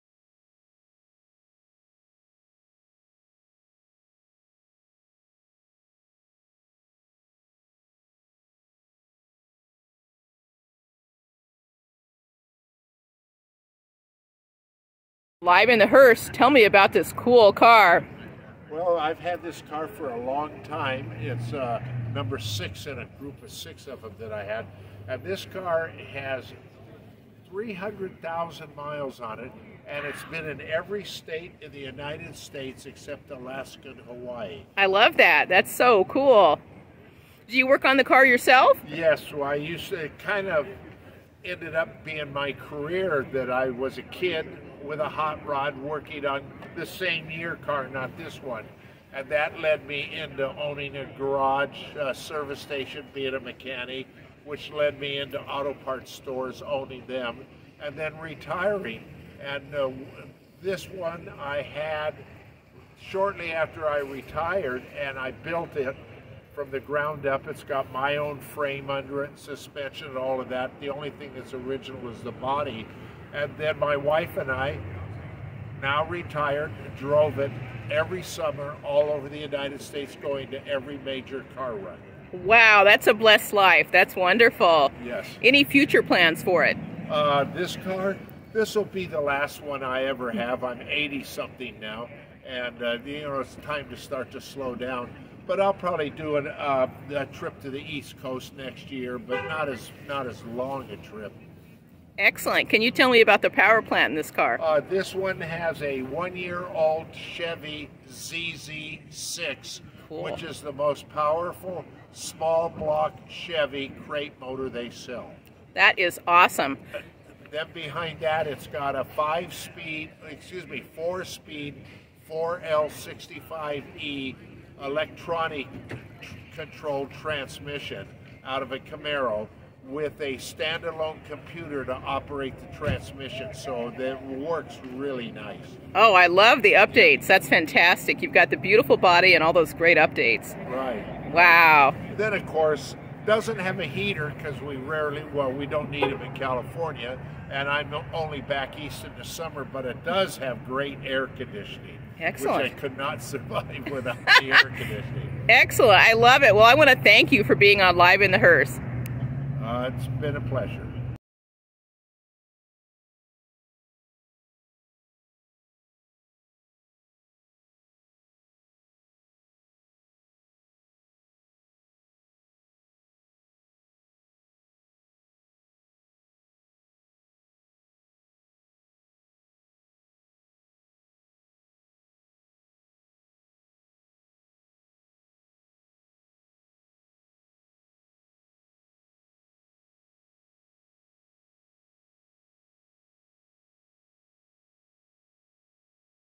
LIVE in the Hearse! Episode #94, 1934 Ford
Hello, Classic Hot Rod Fans,
This 1934 Ford is gorgeous and is a driver. The owner and builder will give you a glimpse of where he and wife have gone in this incredible Ford.